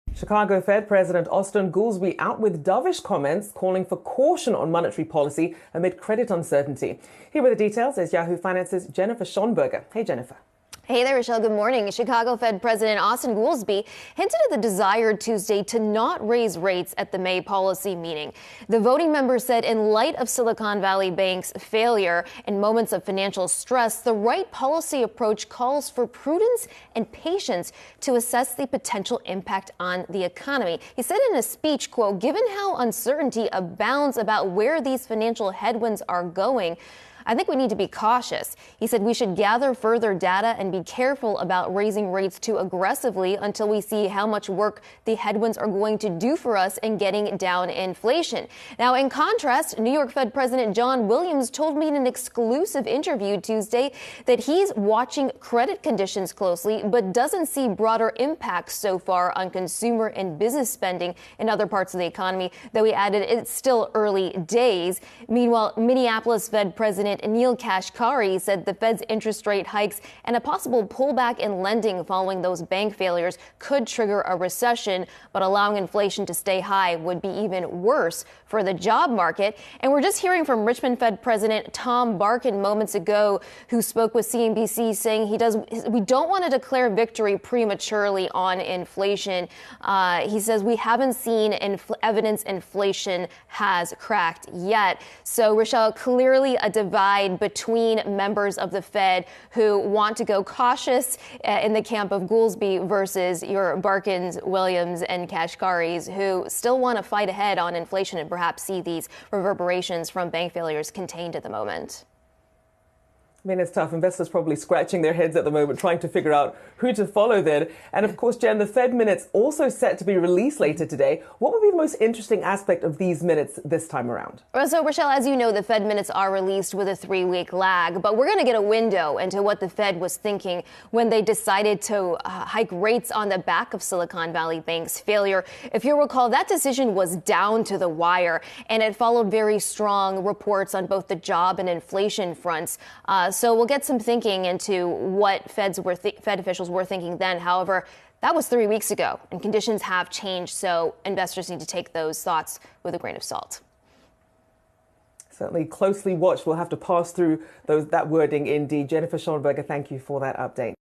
Chicago Fed President Austan Goolsbee warns against further rate hikes
Yahoo Finance’s Jennifer Schonberger joins the Live show to discuss Chicago Fed President Austan Goolsbee’s warning against a further rate hike at May’s FOMC meeting and the expectations for March's Fed minutes.